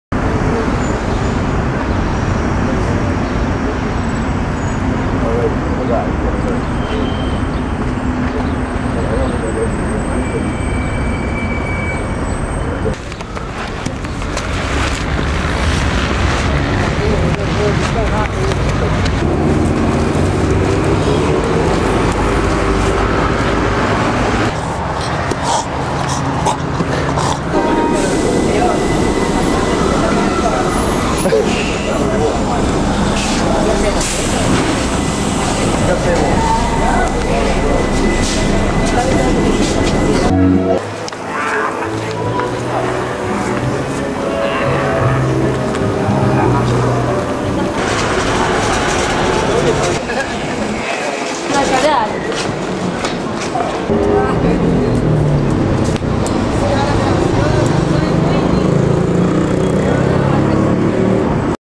ZOMBIE INVASION! THE WALKING DEAD STRIKES BUENOS AIRES 10:30AM
"The Walking Dead" invades major cities worldwide.
*footage courtesy of FOX International Channels